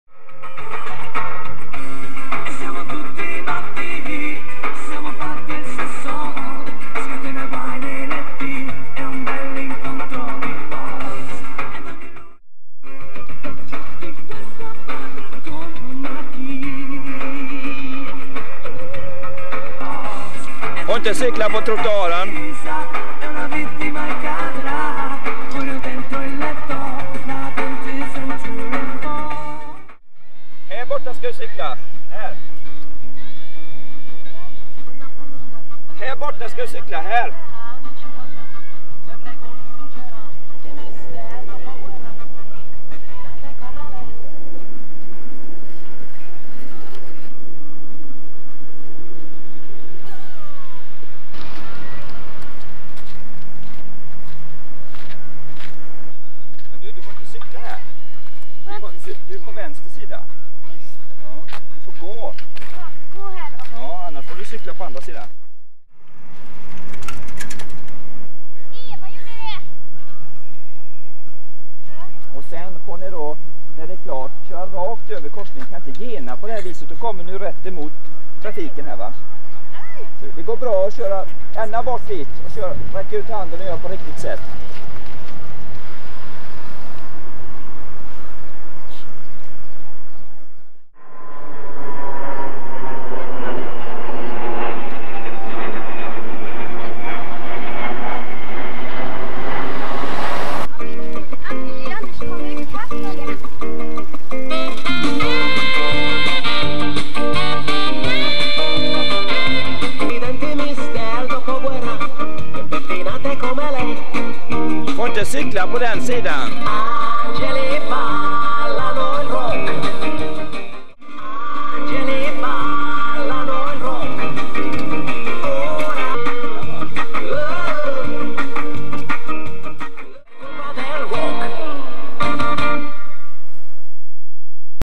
1990 Polis trafiklektion
Jag för länge sen med en trafiklektion i Hindås